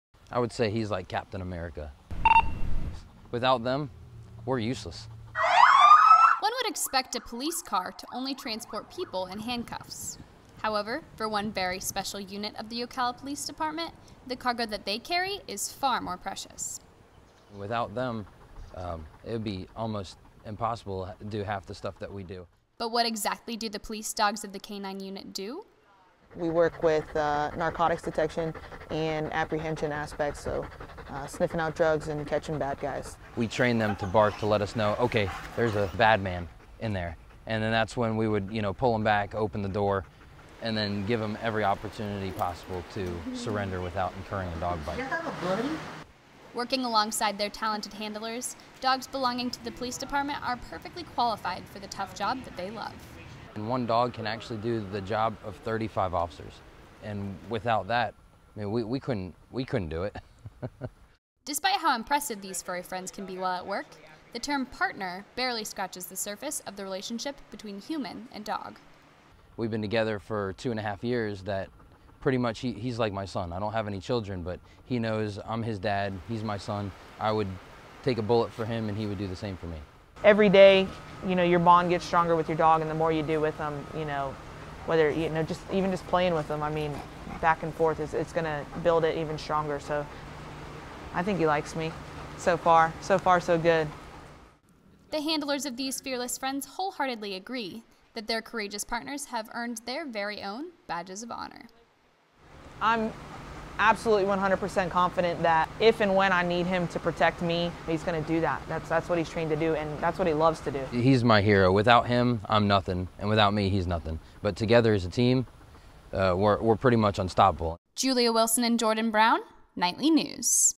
2015 FAME Competition - Courageous K9s
A competition entry produced by Vanguard High School for the 2015 Marion County Student Media Festival.